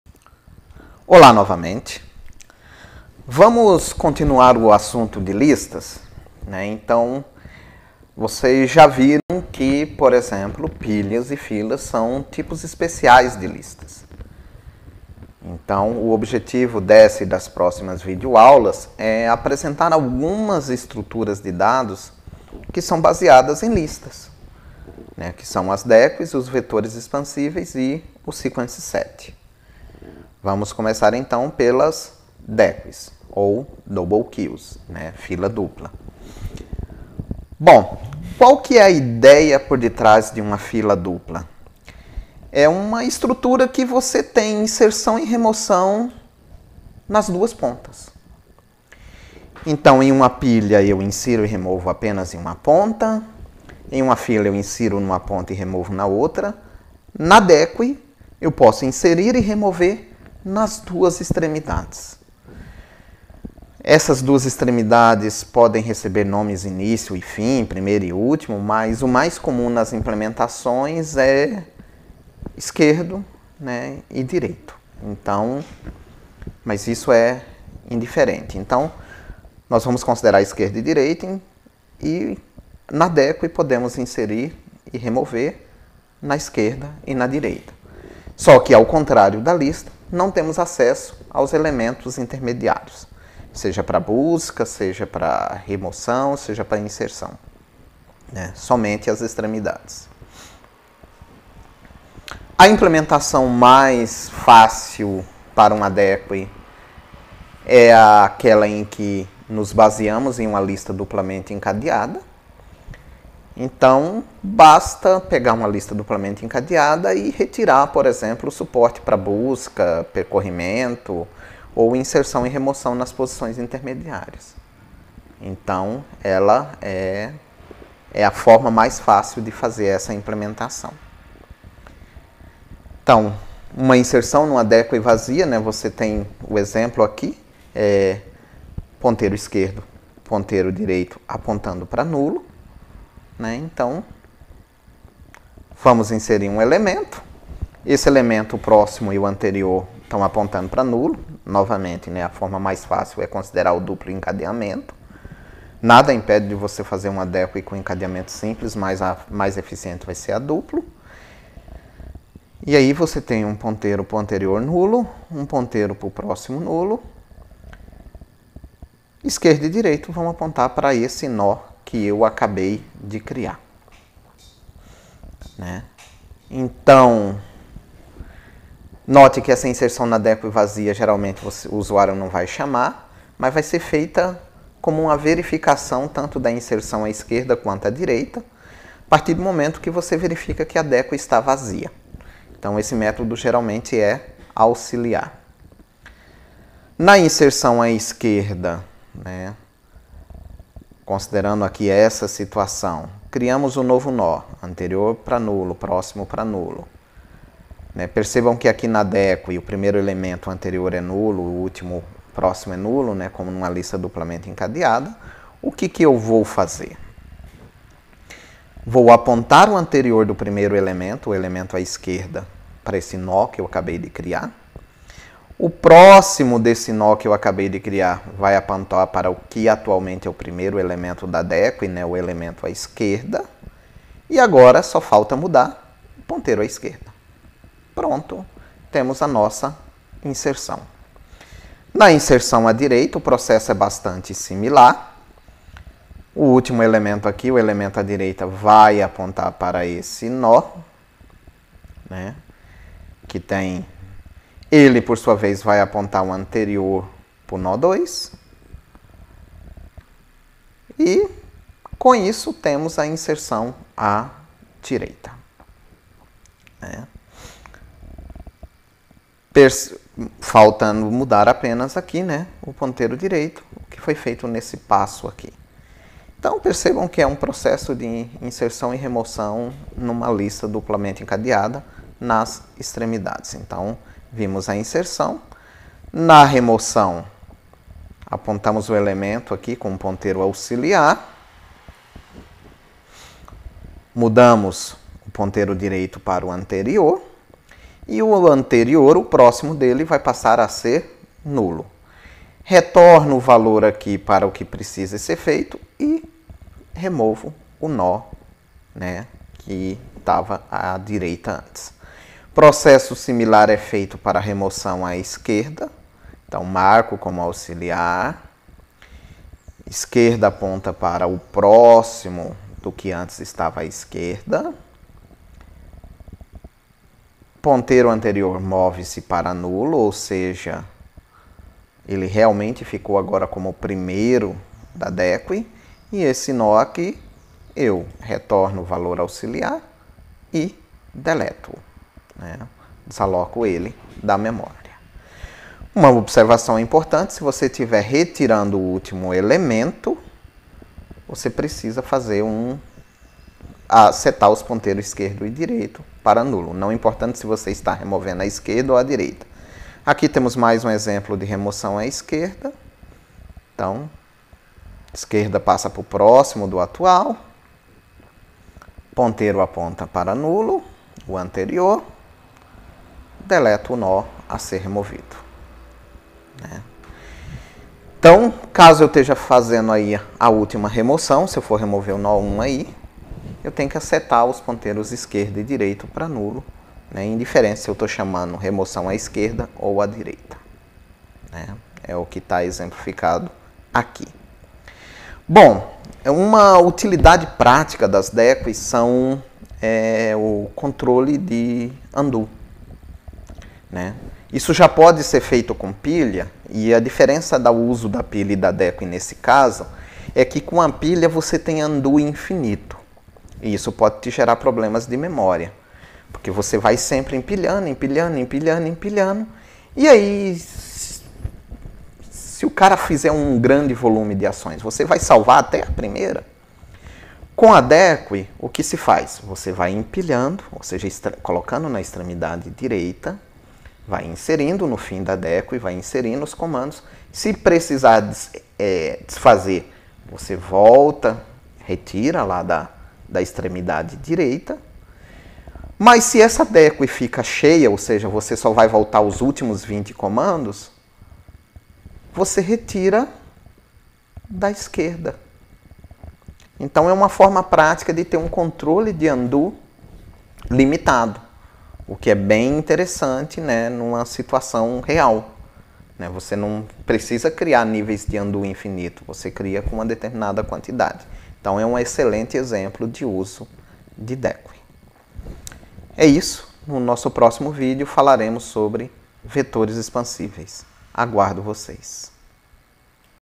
Estruturas de Dados - Aula 10 - Parte 1 - Deque
Apresentação da estrutura de dados fila dupla (double queue - deque)